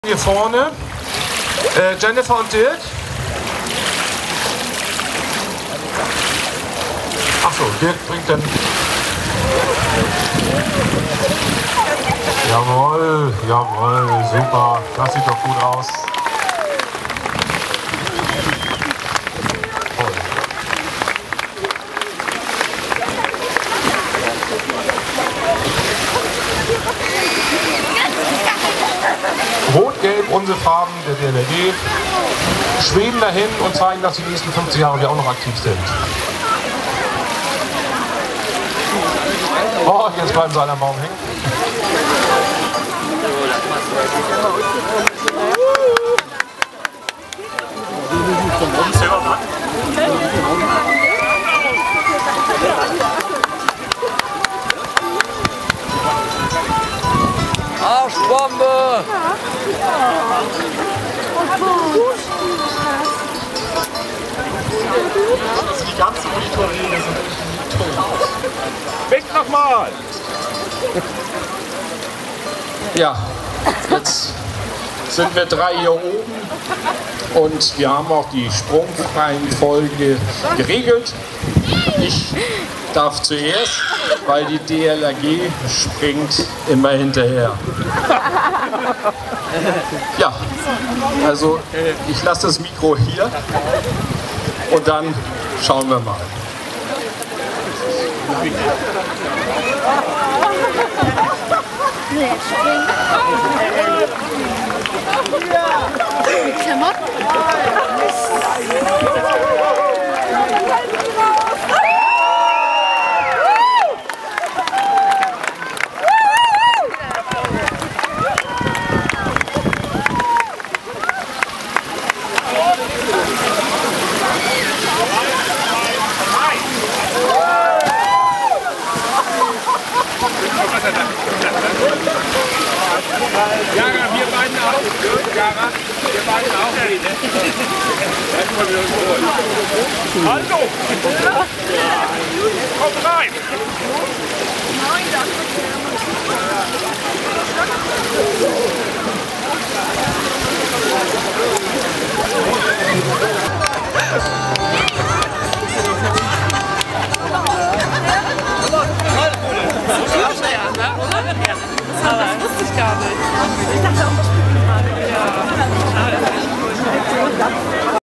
50 Jahre DLRG Neu Wulmstorf / Stellvertretender Bürgermeister springt vom 5m Turm
Die DLRG Neu Wulmstorf feiert ihre 50 Jahre in einem Freibad.

Es wurde gegrillt, es wurden Reden gehalten und der Stellvertretende Bürgermeister Thomas G. Springt im Anzug vom 5m Turm.

Bei Fragen oder Problemen einfach uns auf unserer Homepage oder hier auf Youtube anschreiben.